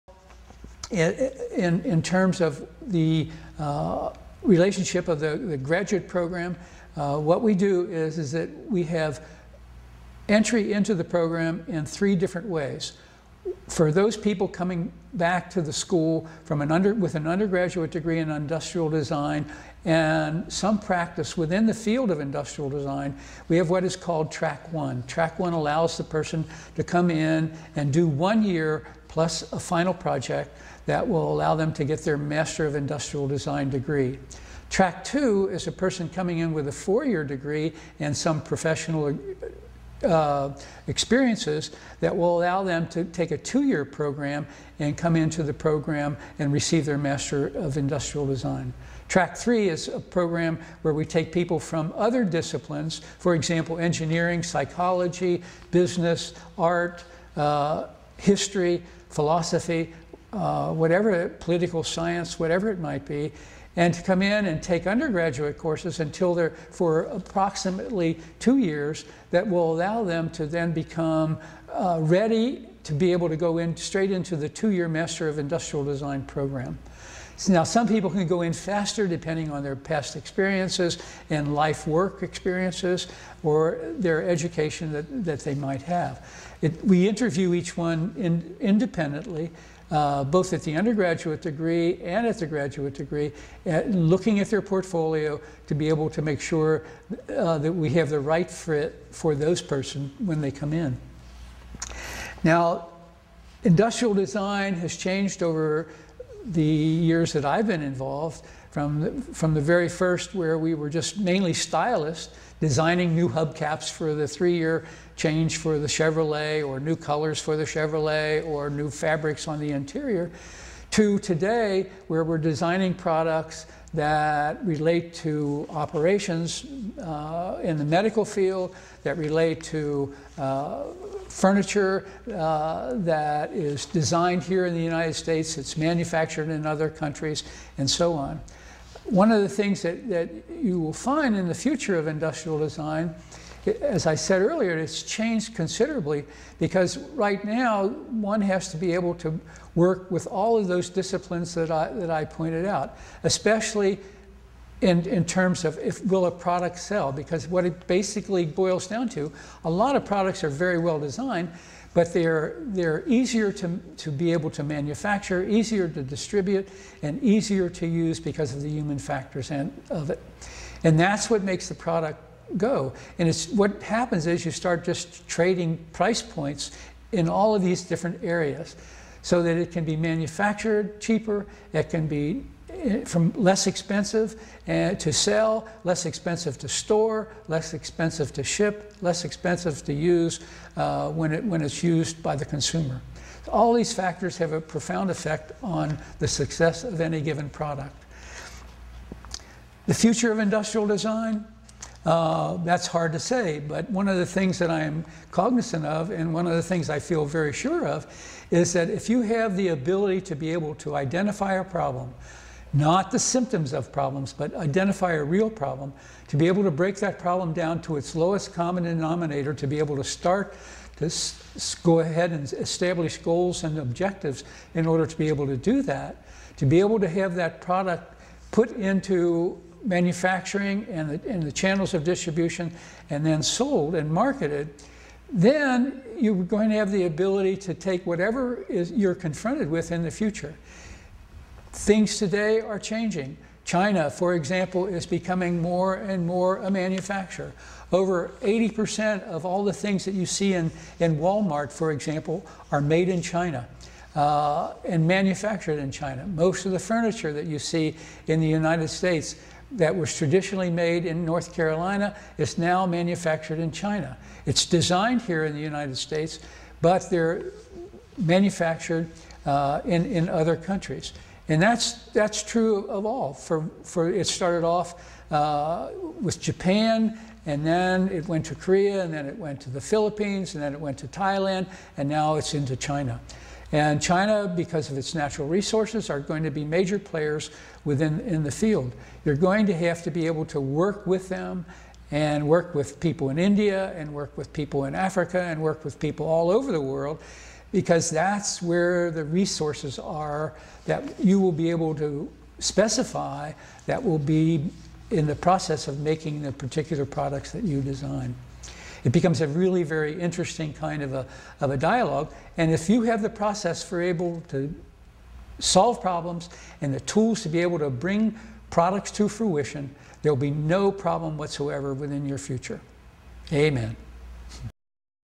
NCSU College of Design - Industrial Design
NC State University, College of Design

Professor Emeritus Vince Foot covers the Master of Industrial Design program and the various tracts, the Industrial Design professional landscape and future trends of Industrial Design.